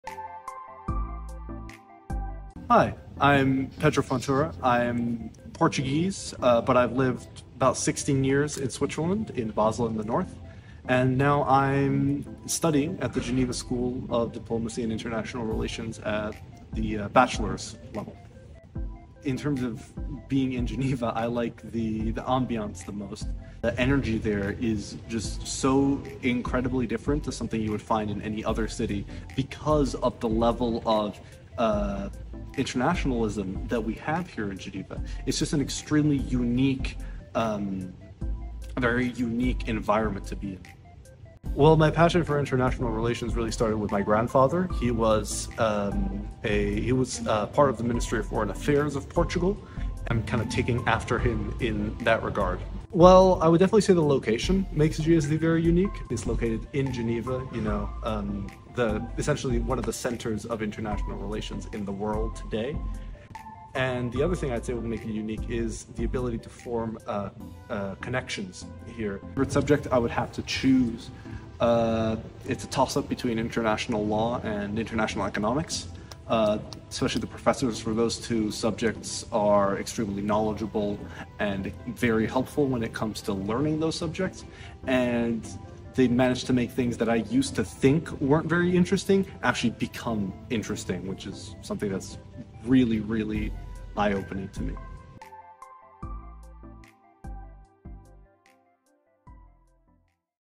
Student Experience: Part 2 (Mr. Pedro Fontura)
Meet our Bachelor student, Mr. Pedro Fontura. Discover his experience as he takes us on a journey through his learning experience at GSD. Watch and uncover his favorite moments and what keeps him motivated! 📚💡